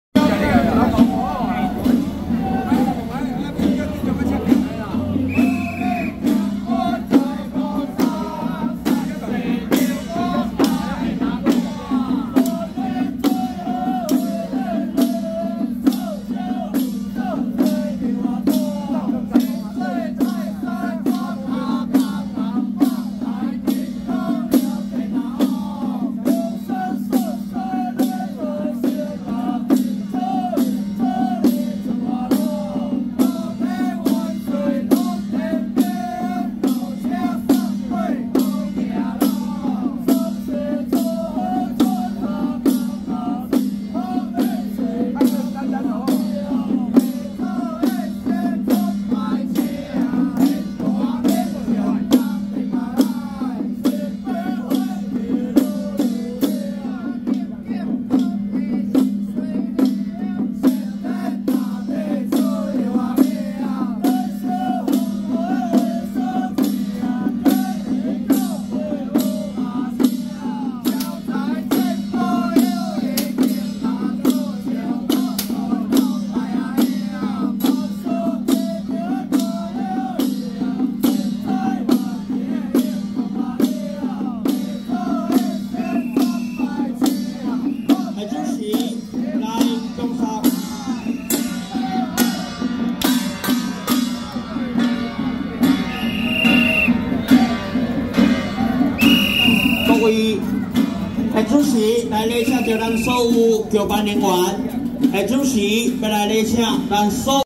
【台北石齋堂心福壇-小法團】~113士林湳雅庄福安宮建宮280週年暨湳雅永安社165週年祈安遶境
台北石齋堂心福壇-小法團